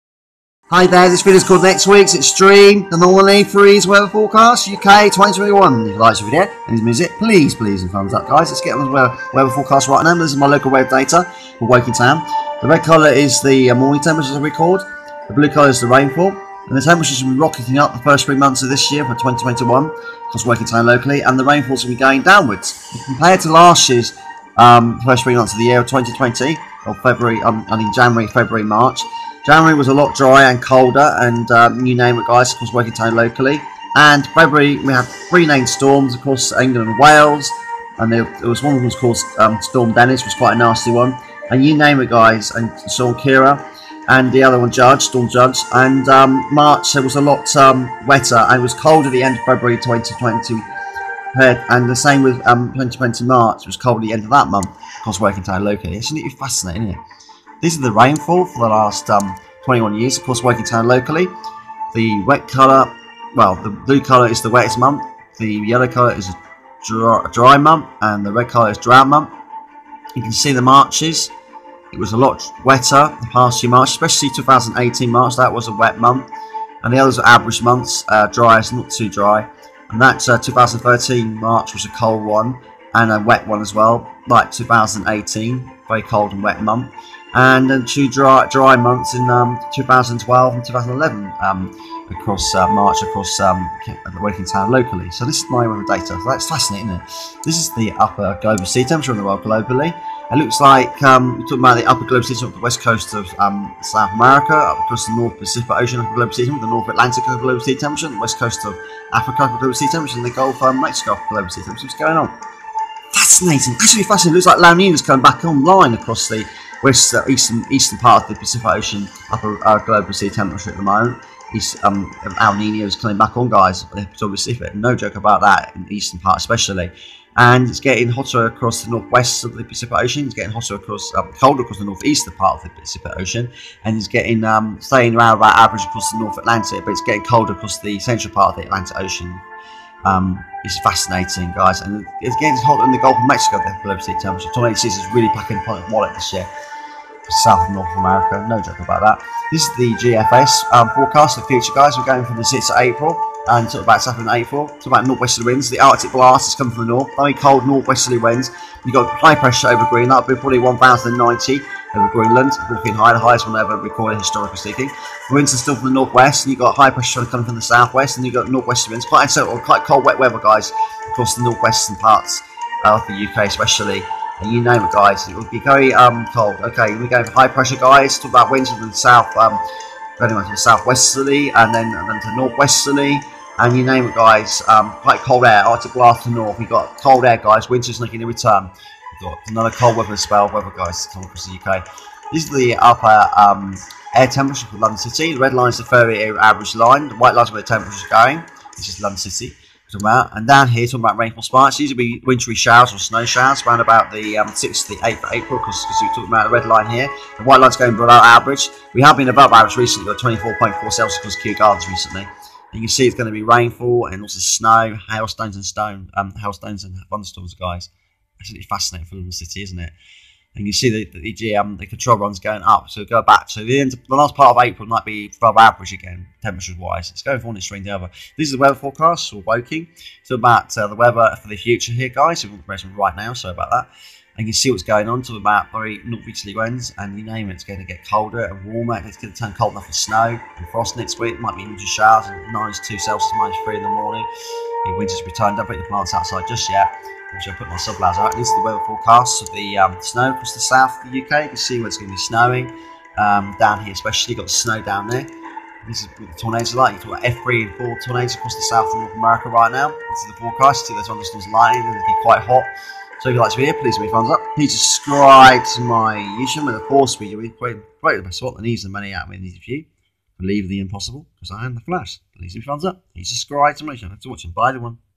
NEXT WEEK'S EXTREME ANOALY FREEZ WEATHER FORECAST UK 2021.
TALK ABOUT EXTREME VOLATILE RISK OF HEAVY SNOW SHOWERS TO THUNDESTORMS. AND HAILSTONE SHOWERS ACROSS THE WHOLE OF THE UK THIS COMING NEXT WEEK. AND THE UPPER GLOBAL SEA TEMPERATURE AS WELL NO JOKE.